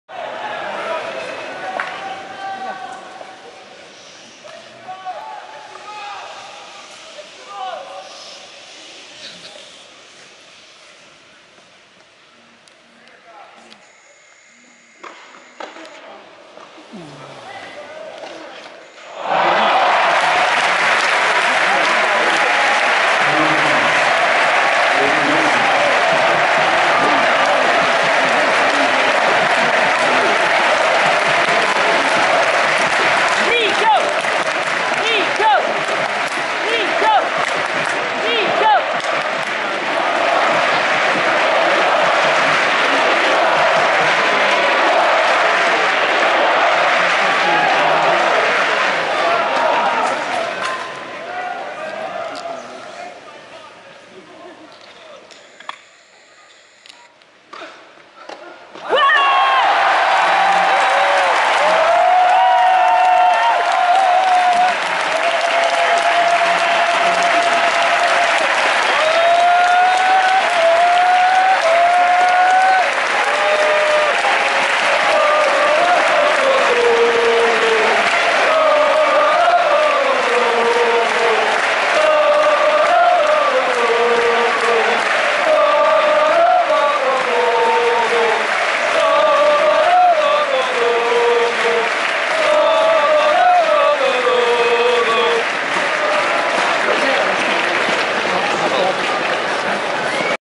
2018 France, Lille, Coupe Davis, Finale, Double, #4, Balles De 2ème Set France
Second St, Balles de Set, Remporté Par la France .

Merci et A+ ...